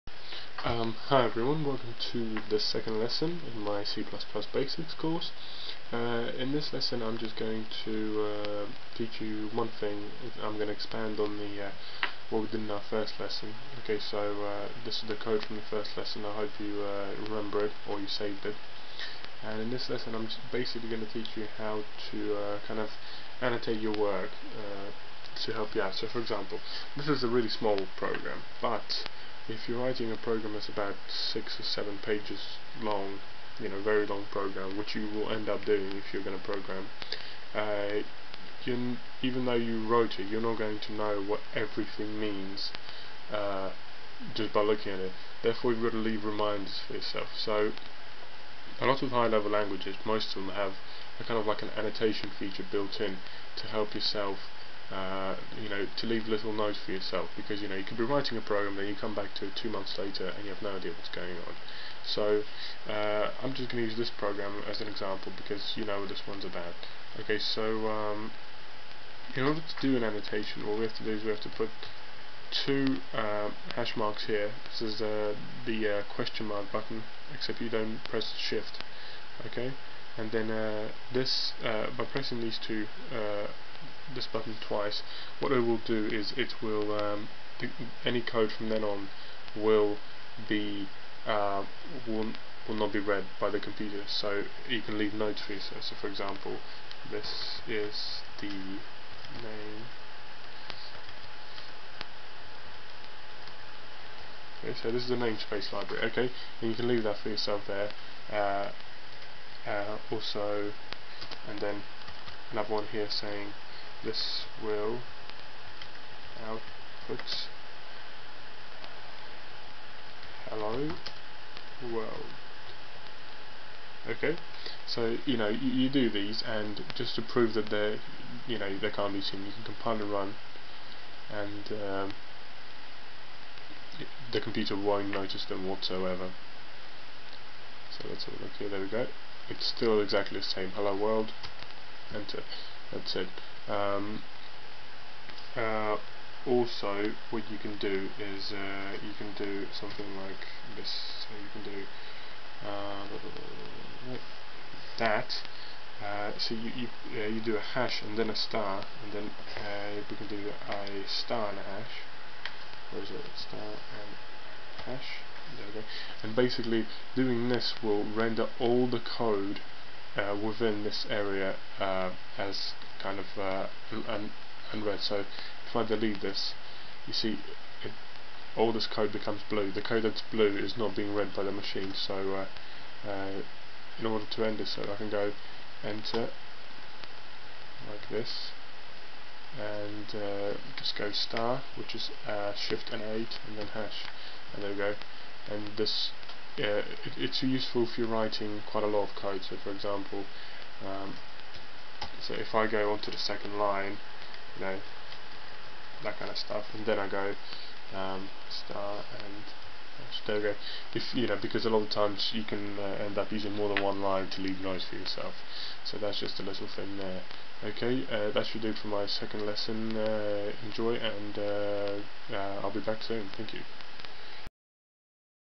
C++ Basics Tutorial 2: How to Annotate
in this lesson we will learn how to leave notes for youreslf while programming through annotation, a very useful techneque